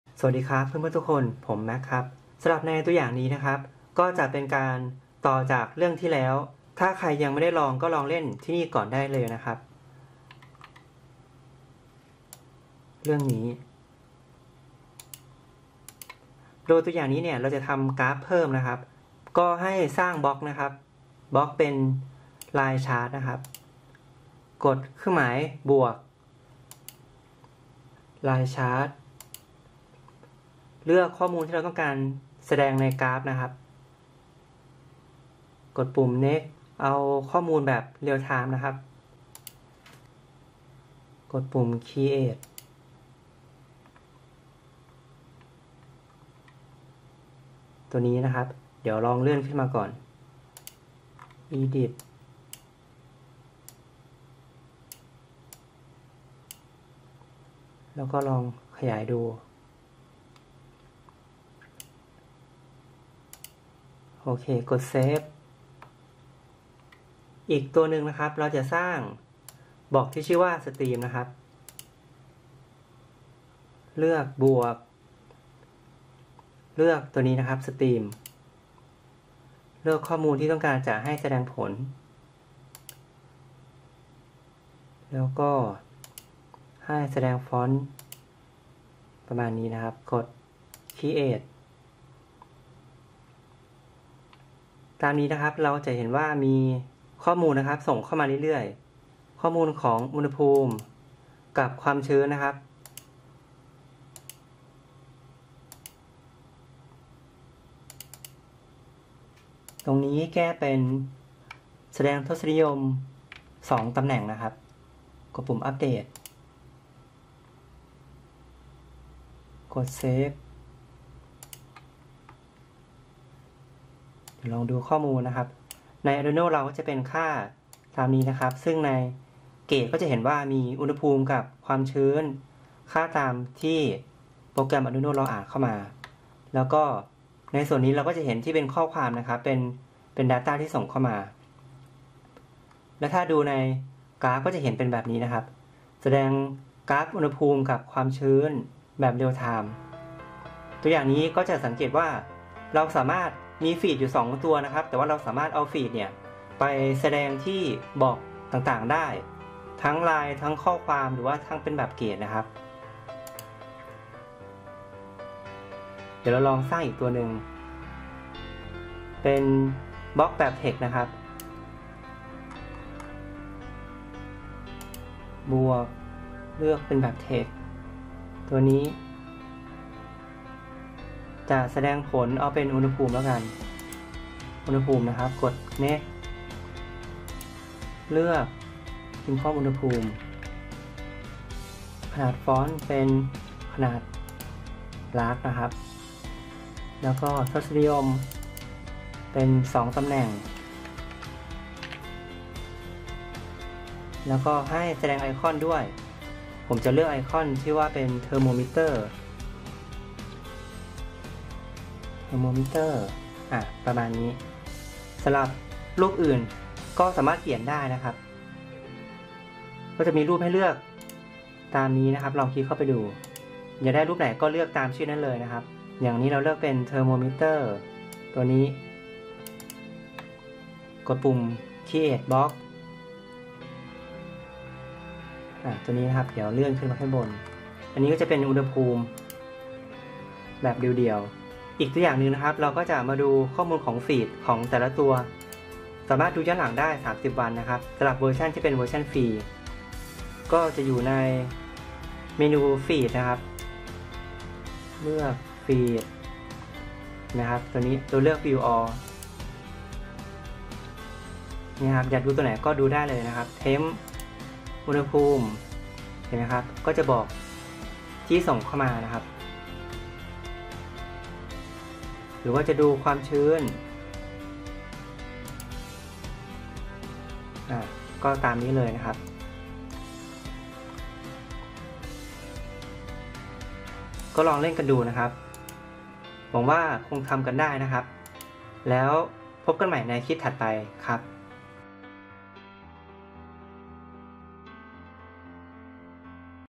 #8 สอนวิธีใช้งาน ESP8266 Adafruit IO ทำเครื่องวัดอุณหภูมิและความชื้นออนไลน์แบบกราฟ
Adafruit IO NodeMCU ESP8266 ตัวอย่างนี้ เราจะทำกราฟเพื่อแสดงค่าอุณหภูมิและความชื้นเพิ่มเติม เพื่อให้ดูได้ง่ายขึ้น และแสดงแบบข้อความที่ส่งมาทั้งหมด